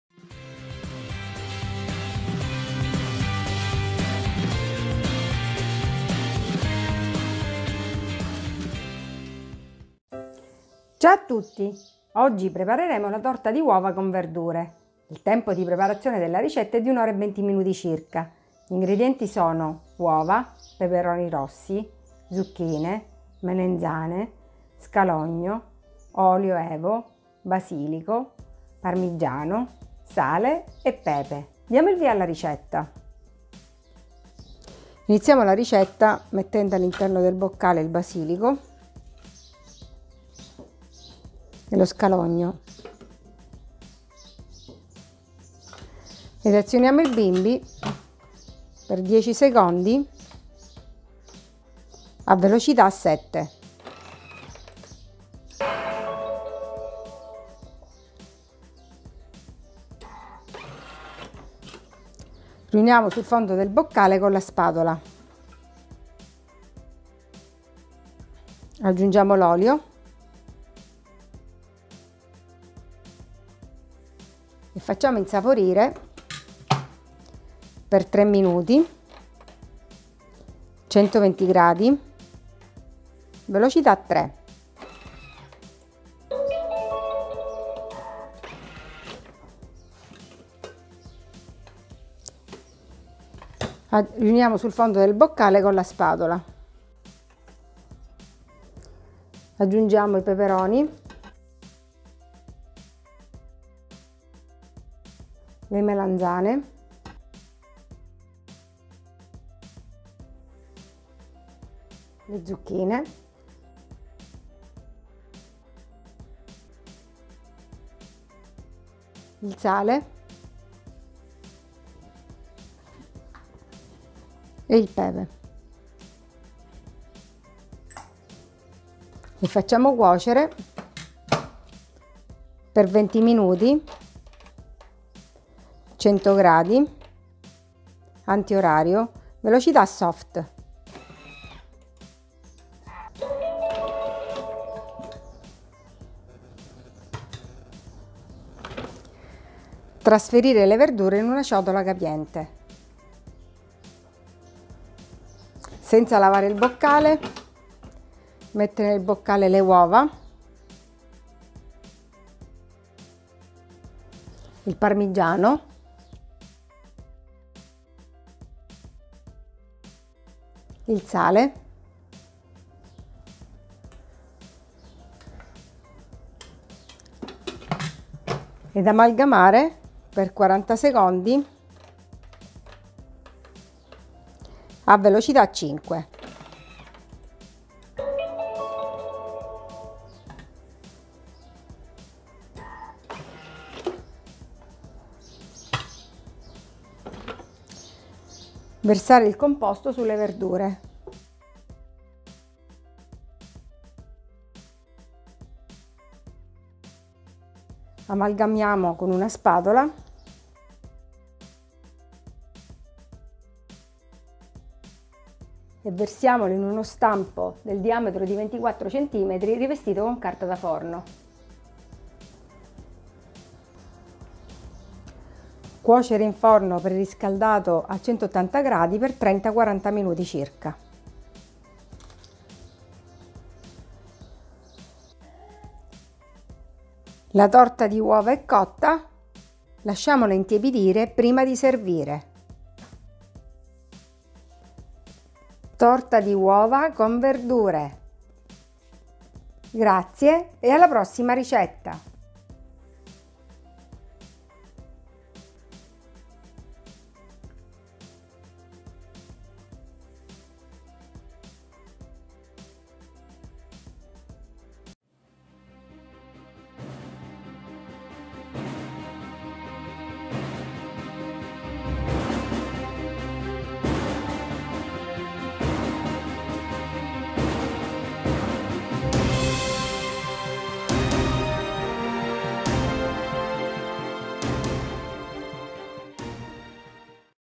Torta di uova con verdure per bimby TM6 TM5 TM31
Ingredienti:
6 uova
230 g di peperoni tagliati a pezzetti
200 g di zucchine tagliate a dadini
350 g di melanzane tagliate a dadini
1 scalogno
40 g di olio e.v.o.
1 mazzetto di basilico fresco
50 g di parmigiano grattugiato